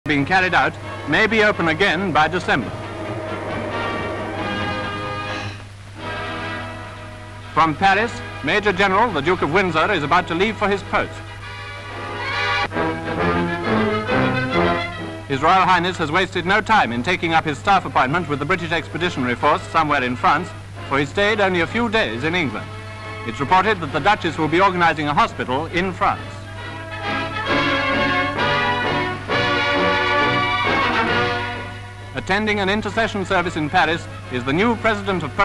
Duke Of Windsor In France
(9 Oct 1939) Major General the Duke of Windsor takes up his staff appointment with the BEF in France.